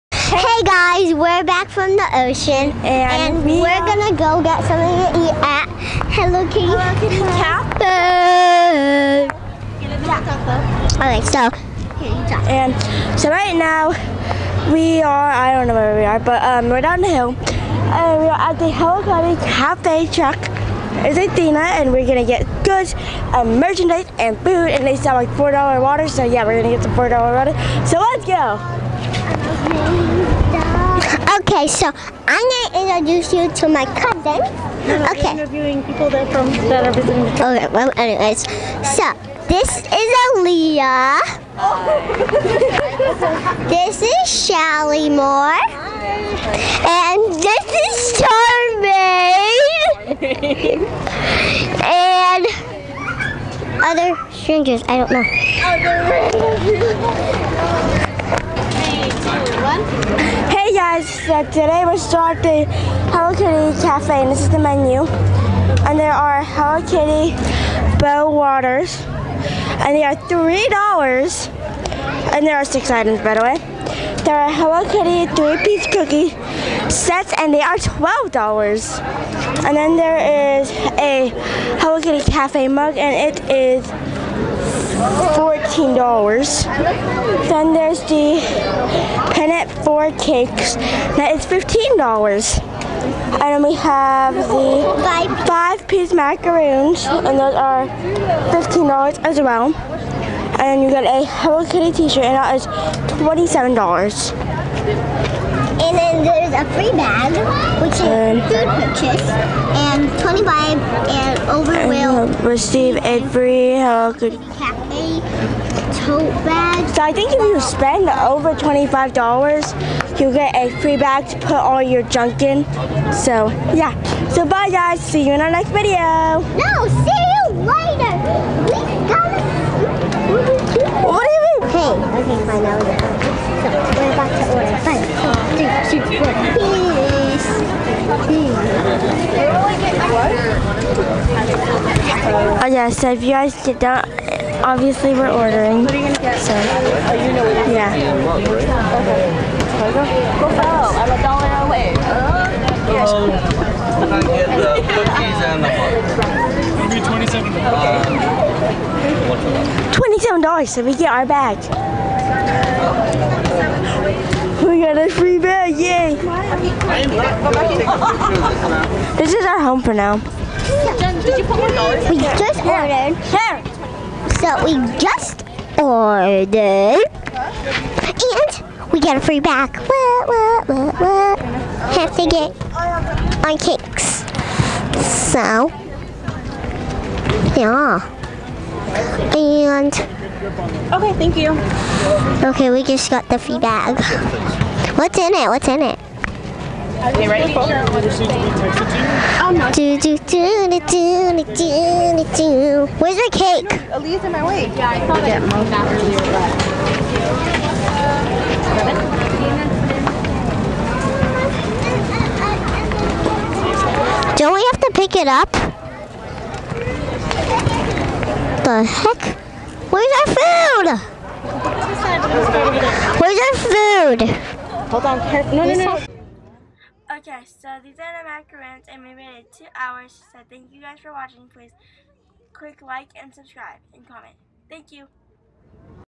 Hello Kitty Pop up Cafe
Visited Hello Kitty Pop Up Cafe.  We waited 2 hours in line to buy Macarons and Water.

We also meet up with a few of our fans.